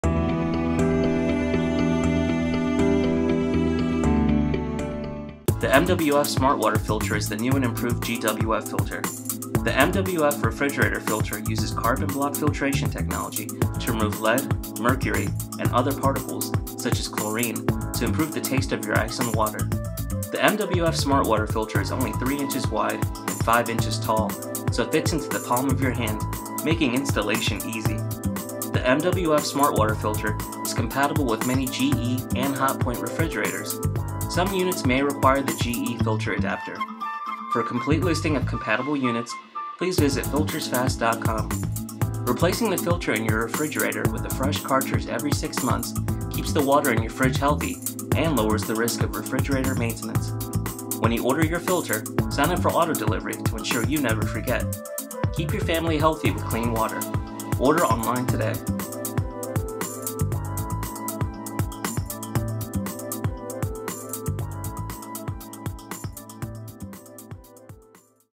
1 Min. Overview: MWF SmartWater Refrigerator Filter by GE
Reduce the amount of harmful particles in your refrigerator water.

Video Script

The mwf smartwater filter is the new and improved GWF Filter.

The mwf refrigerator filter uses carbon block filtration technology to remove led. mercury... and other particles such as chlorine to improve the taste of your ice & water.

The mwf smart water filter is only 3 inches wide and 5 inches tall so it fits into the palm of your hand. making installation easy.

The mwf smart water filter is compatible with many GE and hot point refrigerators.
Some units may require the GE filter adapter.

For a complete listing of compatible units, please visit filtersfast.com.

Replacing the filter in your refrigerator with a fresh cartridge every 6 months keeps the water in your fridge healthy and lowers the risk of refrigerator maintenance.

Keep your family health with clean water.

Order online today!